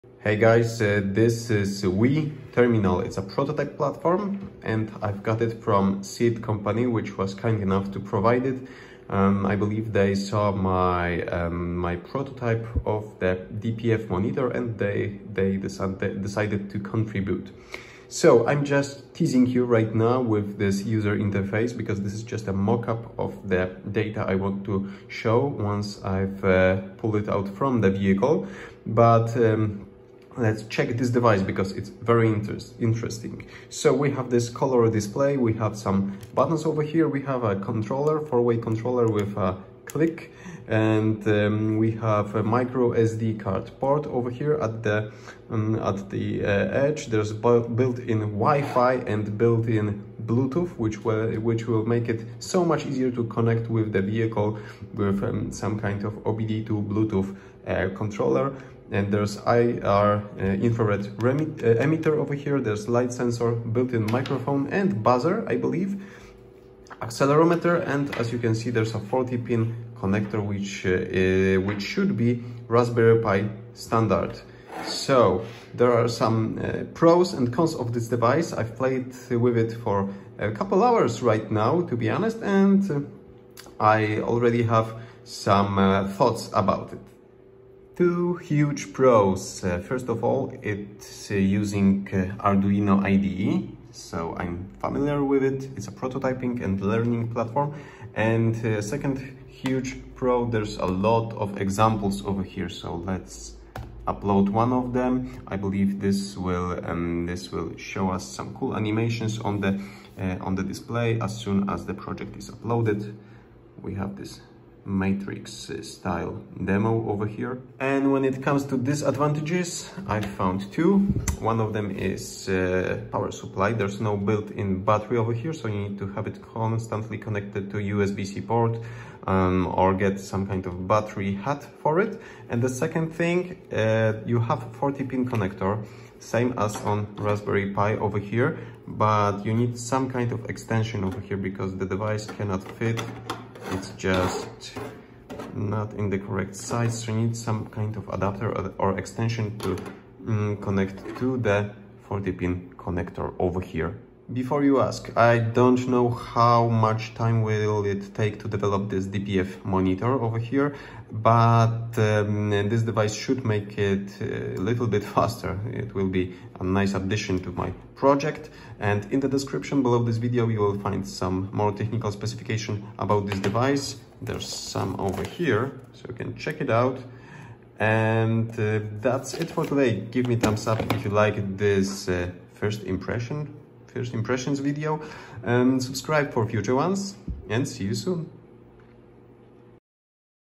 Wio Terminal first impressions - prototype platform demo for DPF monitor
Planning to make DPF monitor to control particulate filter regeneration state and made a teaser some time ago. Seeed company decided to provide their device - Wio Terminal - as a way to contribute to the project. Here are my first impressions after few hours playing around with this cool device.

ℹ️ Since WioTerminal is equipped with Bluetooth and WiFi (as all IoT devices should be), which will make it easier to connect with vehicle via ELM327 Bluetooth scanner to pull data from the vehicle for DPF monitoring. That's instead of hacking directly into CAN-BUS and sniff for data.

👍 Pros
Works with Arduino IDE.
A lot of code examples.
A lot of I/O connections.

👎 Cons
No build-in battery. Needs to be connected to USB-C or requires additional equipment.
40-pin connector requires extension to fit on a Raspberry Pi.

🛠 Tech specs of Wio Terminal D51R
CPU: 200MHz ATSAMD51 with 32MBit flash, running at 120MHz
Display: 2.4 inch 320x240
Bluetooth: BLE 5.0
WiFi: a/b/g/n 2.4G/5G
Connectors: USB-C (client + host), 40-pin GPIO, 2x Grove connector, IR
microSD port (SDHC up to 16GB)
Buttons: 3 user programmable, 4-way joystick, power/reset.
Sensors: 3-axis accelerometer, microphone, buzzer, light

0:00 Wio Terminal
0:10 DPF monitor teaser
0:20 User interface mockup for DPF monitor
0:33 Couple Wio Terminal specs
1:15 Pros and cons after few hours
1:28 Arduino IDE
1:40 A lot of code examples
1:58 Power supply
2:12 40-pin GPIO connector
2:34 Plans for the DPF monitor